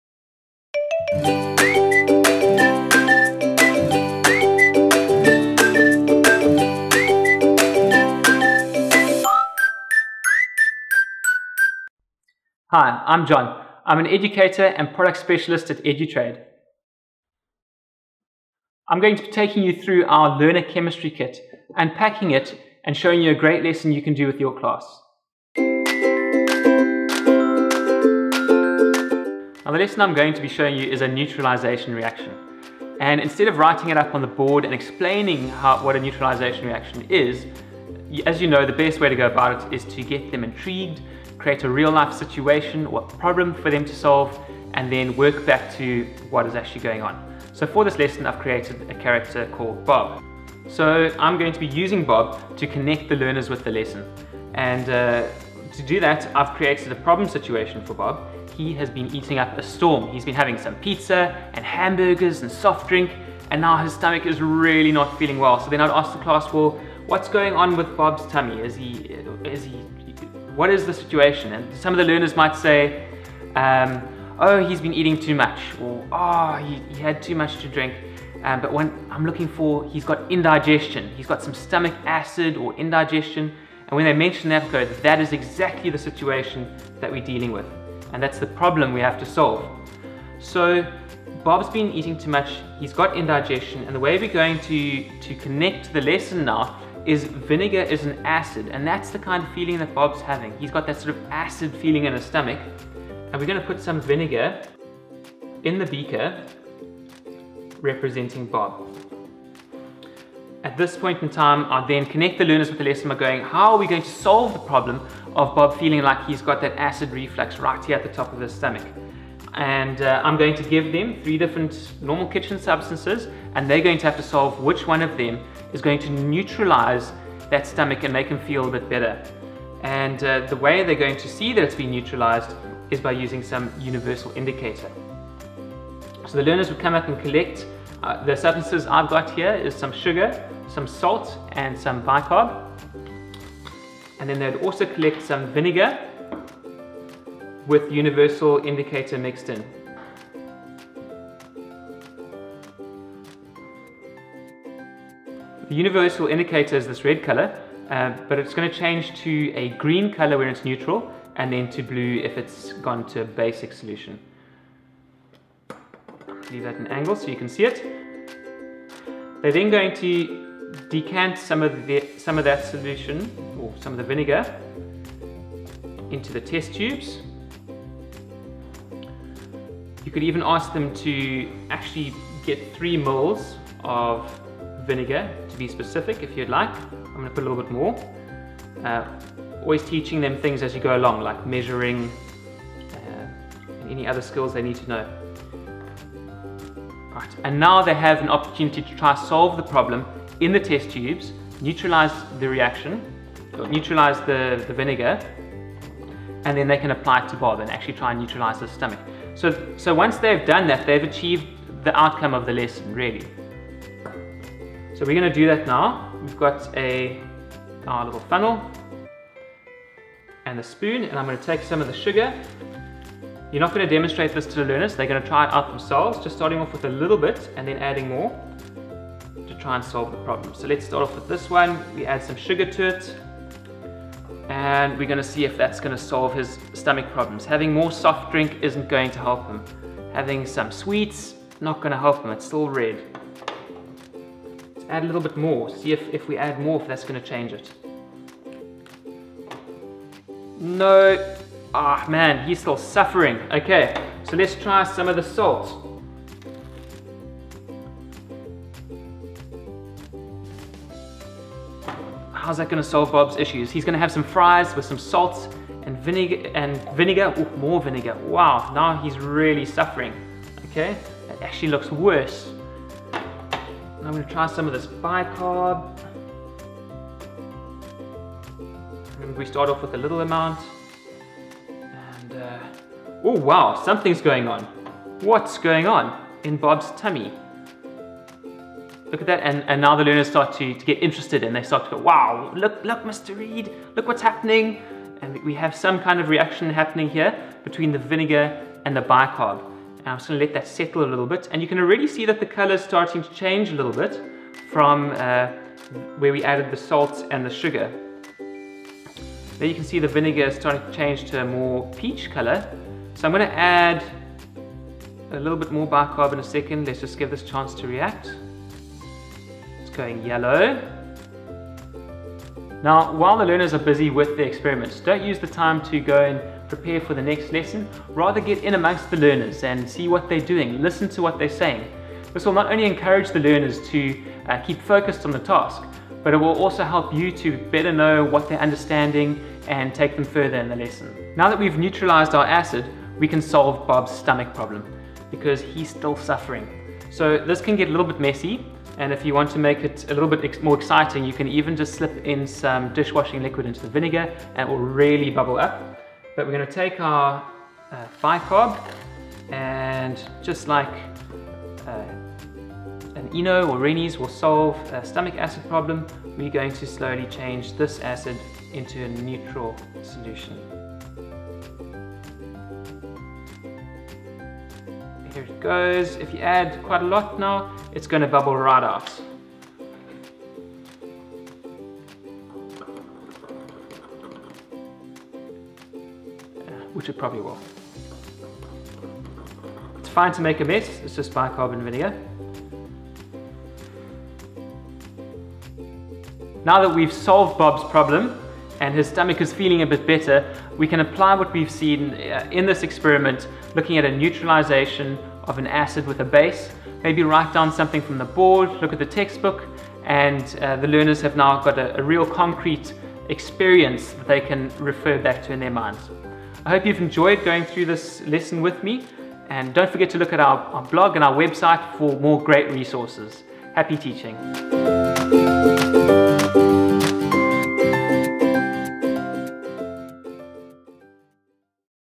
Acid Base Neutralisation Reaction Experiment
An entertaining twist on the classic neutralisation reaction experiment. John will inspire you with creative ideas for your Science lessons.

RESOURCES & LINKS:

"How to get the most from the Learner Chemistry Kit" (Blog)